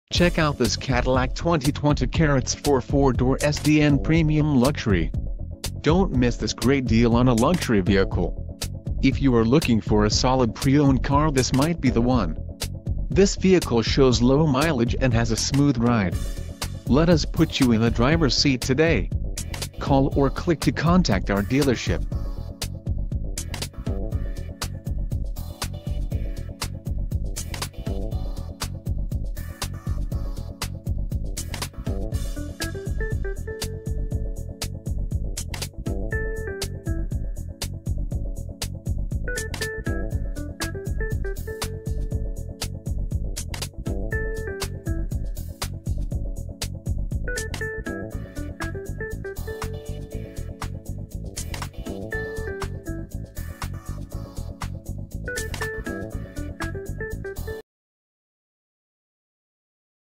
2020 Cadillac CT4 4dr Sdn Premium Luxury - McDonald GMC C...
Certified 2020 Cadillac CT4 4dr Sdn Premium Luxury, satin steel metallic exterior, whisper beige white interior, 4, 2.0l, automatic, awd, 4 doors, 8,099 miles, Stock #15132939, VIN: 1G6DF5RK7L0132939,